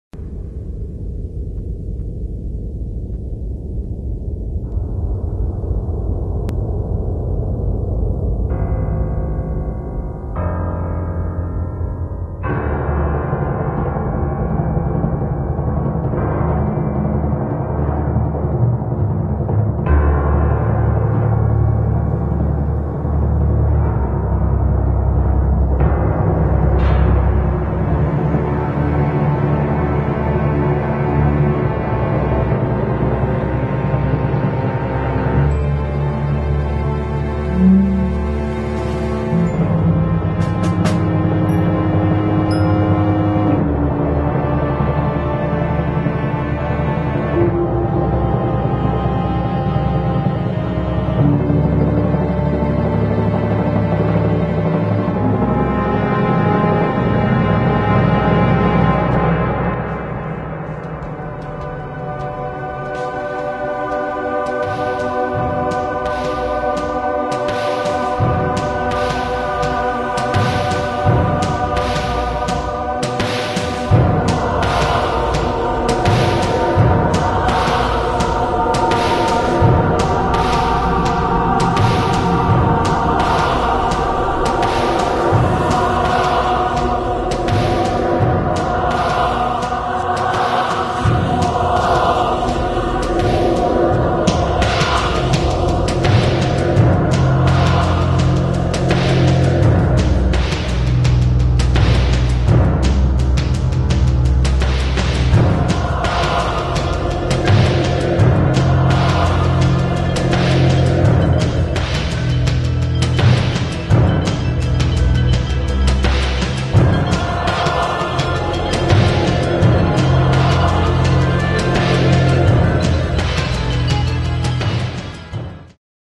Animation Reel - Centipede
Another character from my school's game project. Sorry for the awkward motion blur. Model by Toni Repo and texture by Tuukka Rinkinen.